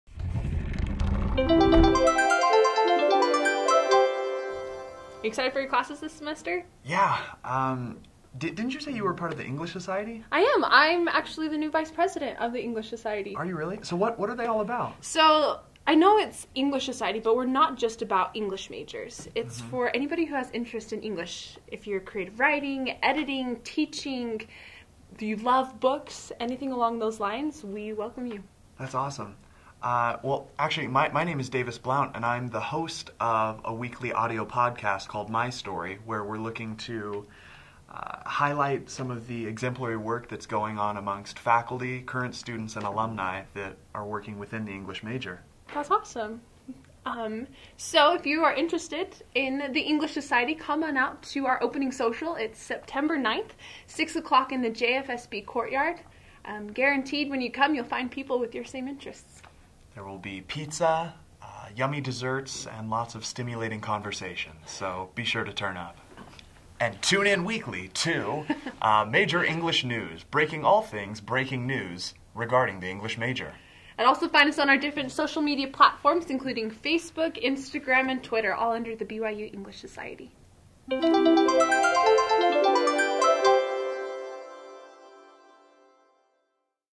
Major English News - Episode 001
The BYU English Society presents its weekly video news magazine for the week of September 1 - 7, 2015. Also listen to our weekly audio podcast, "My Story" on Soundcloud